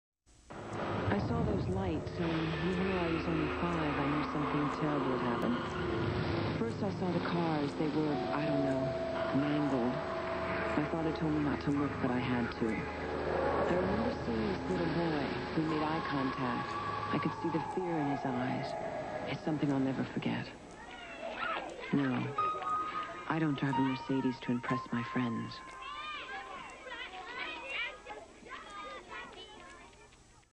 1992 Mercedes Benz Commercial
Mercedes Benz is only carmakers has more safety systems than any luxury car lines. Which is the first carmakers with dual airbags in the W116 S-Class in 1978.

Aired during "NFL Atlanta VS Washington" & "A Repeat of Deadly Machine" on Unknown Station of NBC and CBS on September 13,1992. Uploaded by pannoni 8.